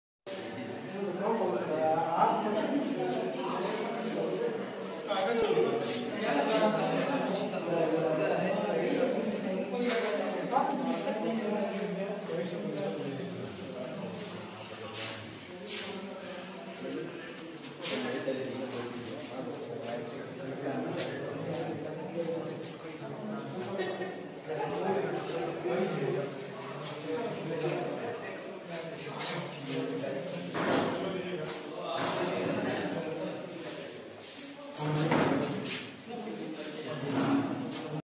chemistry का practical करते हुए
chemistry ka practical karte huye
practical room me
differentiate between acetaldehyde and acetone, test for acetaldehyde, group aldehyde and ketone testing, Functional group analysis, test for ketone.group analysis for organic compounds.,